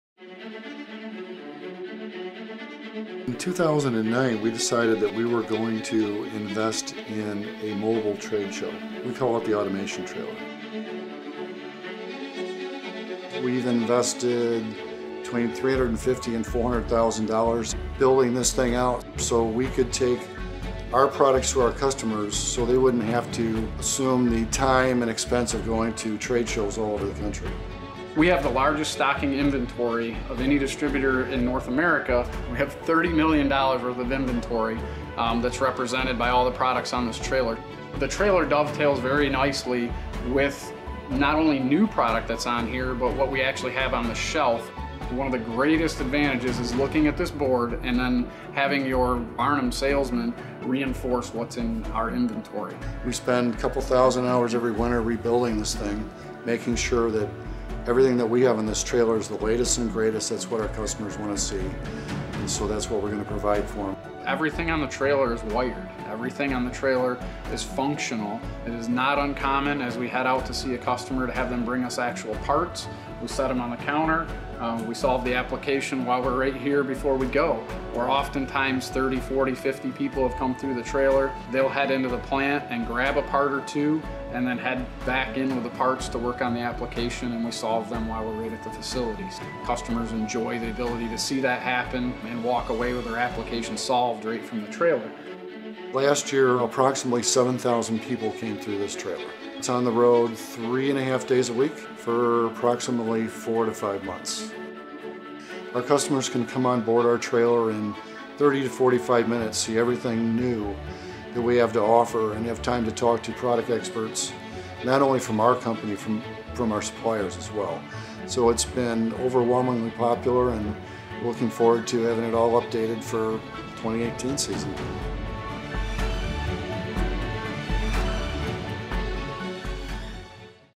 Mobile Automation Tours | H.H. Barnum's Automation Trailer
Our 38-foot automation trailer showcases a wide variety of products for factory automation from all major suppliers. Take advantage of this interactive experience with over 300 working components right at your fingertips!